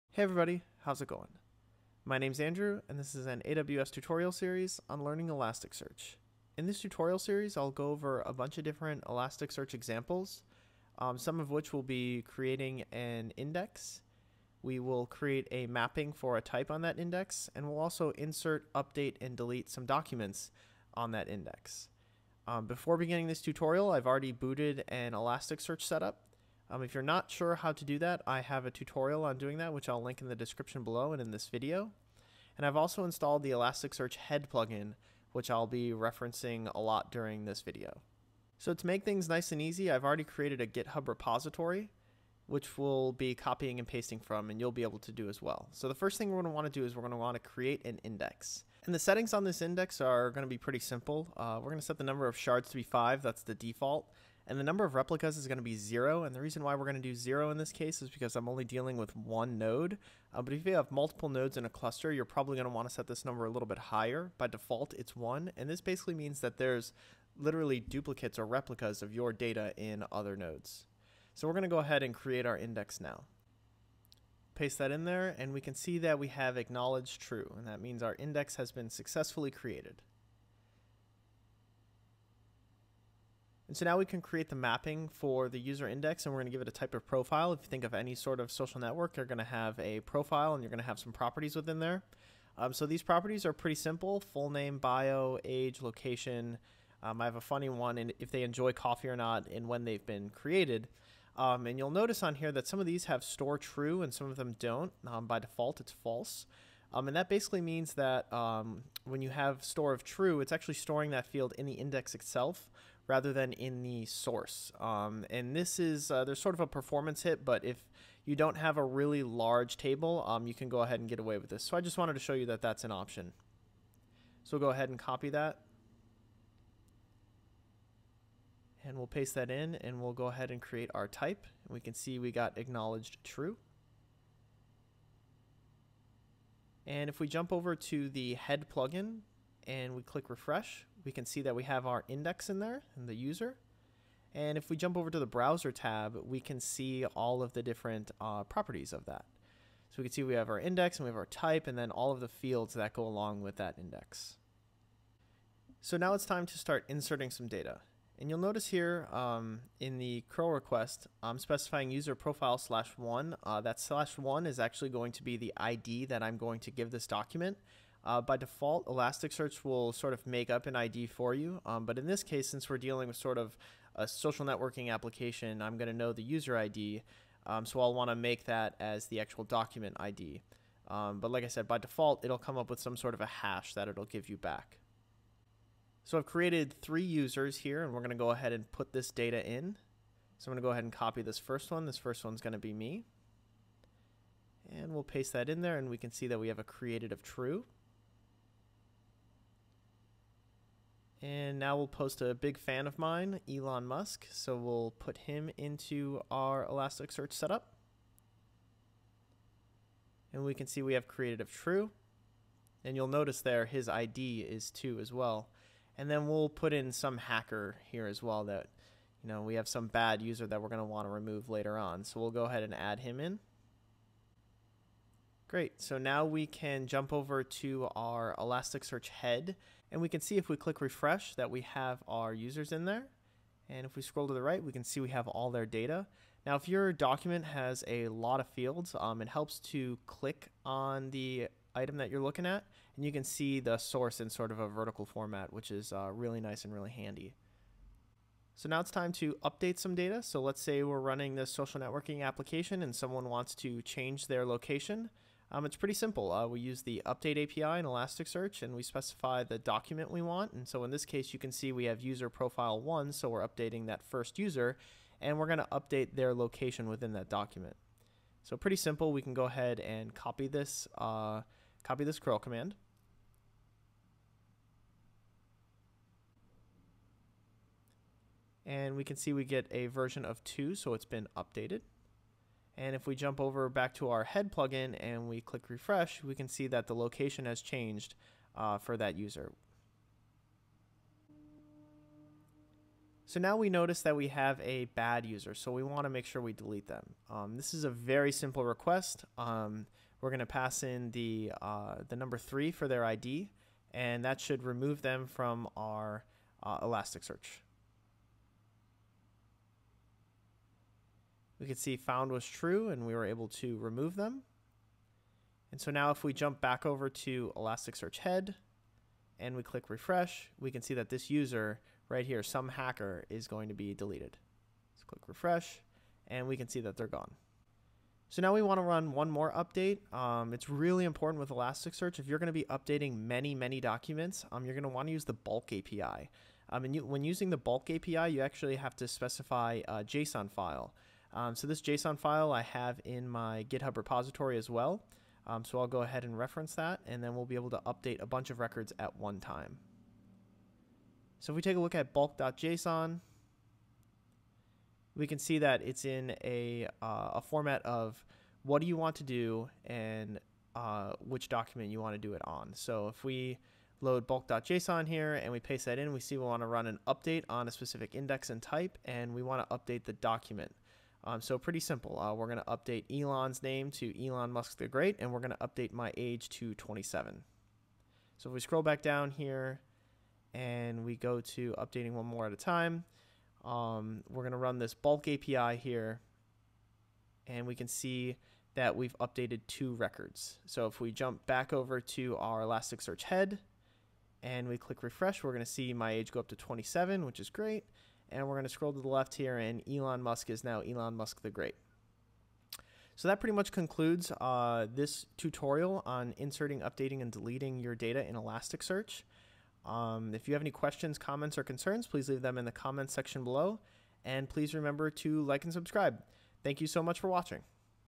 Elasticsearch Tutorial - Insert, Update, Deleting Data [1 of 2]
Elasticsearch is an amazing tool for search but I have noticed there is not many good tutorials to help beginners get started. So I created this to help anyone starting off! Everything is in my github and can easy be copy pasted. To learn how to boot an elasticsearch server I have a tutorial for that as well!

How to setup elasticsearch.